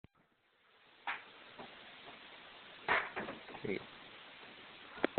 living room basketball t1
awesome off wall behind back shot.